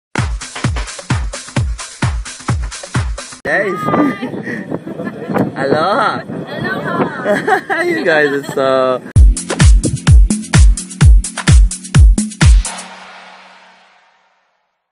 Aloha Party - Adventure Gold Coast - 11:54am Sunday 20th of November 2011
Adventures, parties and great times on Australia's Gold Coast